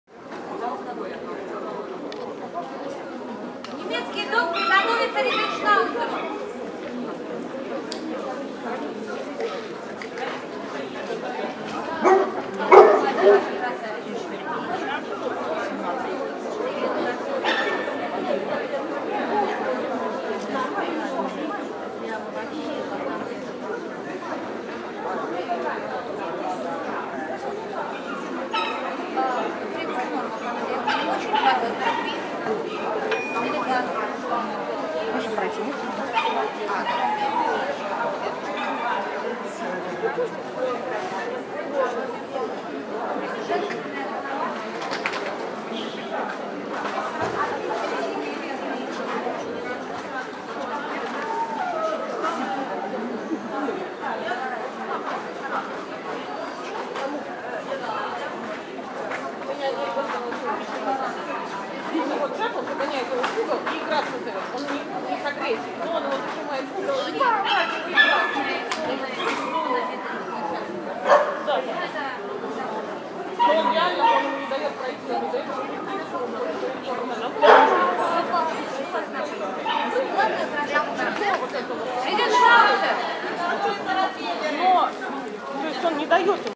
12.11.2022 г. Белгород .Национальная выставка собак всех пород САС - ЧФ "ОСЕННИЕ ВСТРЕЧИ"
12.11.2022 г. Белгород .
Национальная выставка собак всех пород САС - ЧФ
"ОСЕННИЕ ВСТРЕЧИ"
Немецкий дог
Эксперт: Сенашенко Е.
CHELITA IZ TSARSKOSELSKOY YSADBY (Лиза в 17 мес.)
Отлично, САС, ЛПП, BIG-3
зав. Елена Андреева
вл. Бондаренко О.